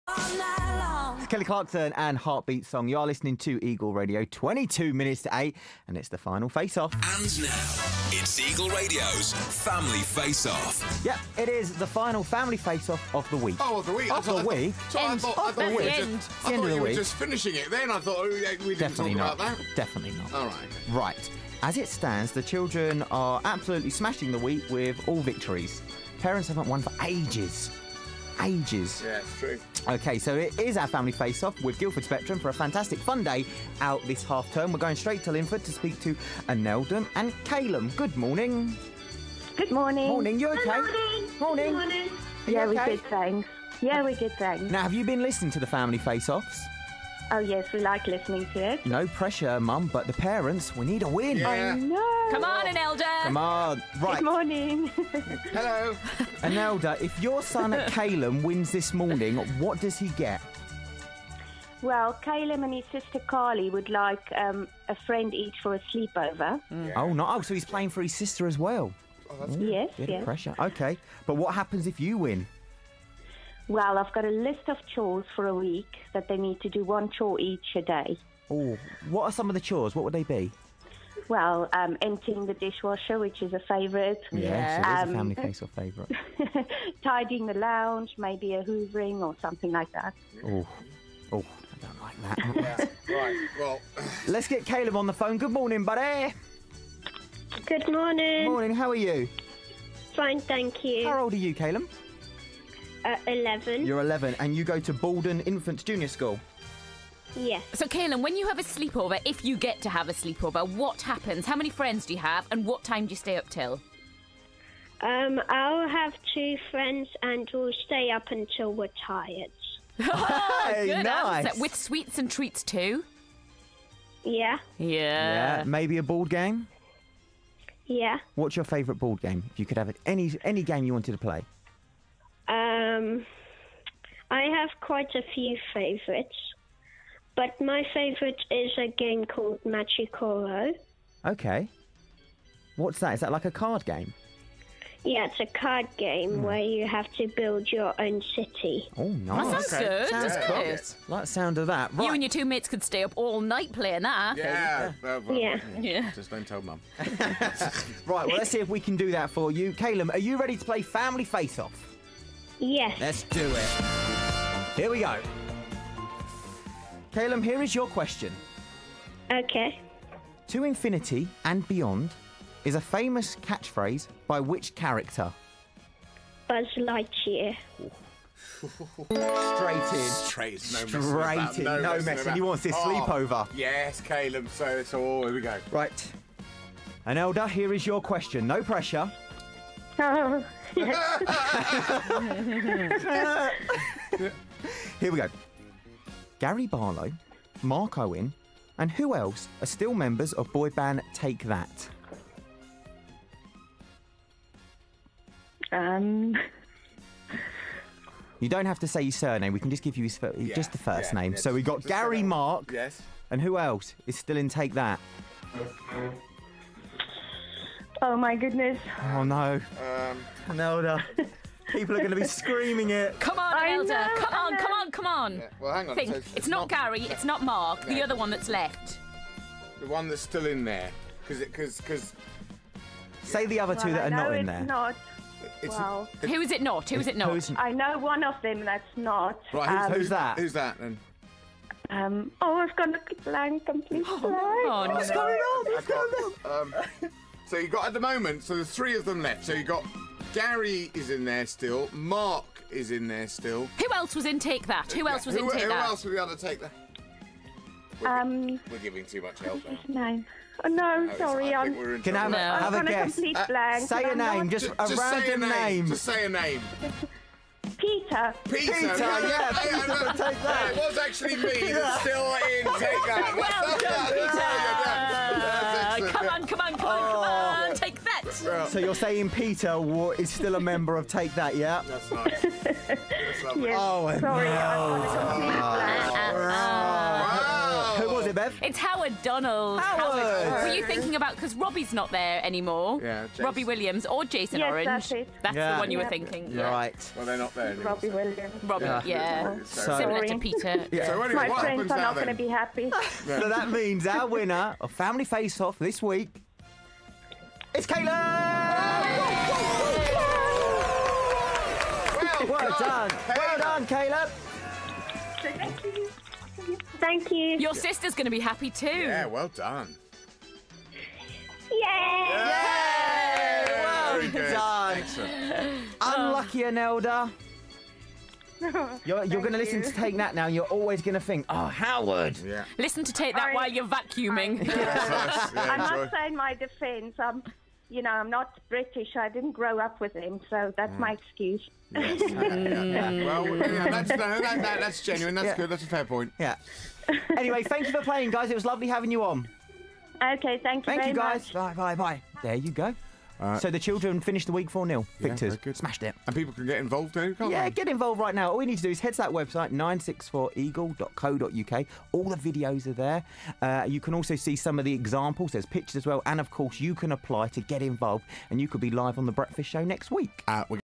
Family Faceoff - Anelda & Caelum
Final Family Faceoff of the week! Who would win mum Anelda or son Caelum?